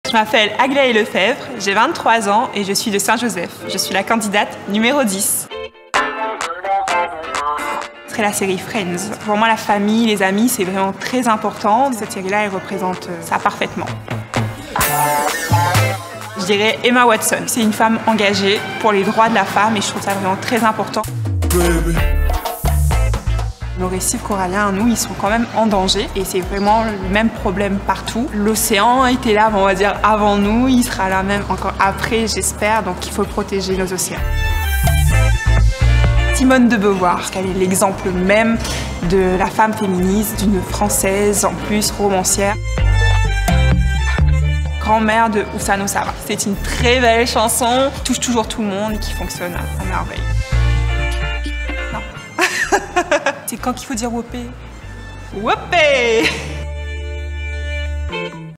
Aglaé Lefevre - Candidate n°10 - Miss Réunion 2021
CANALBOX ❤ MISS REUNION
Découvrez le portrait de Aglaé Lefevre, Candidate n°10 à l'élection Miss Réunion 2021😍
Quelle est sa cause environnementale ? La femme qui l'inspire le plus ?
Aglaé nous dit tout
CANALBOX, partenaire officiel Miss Réunion 2021
Wopé Comité Miss Réunion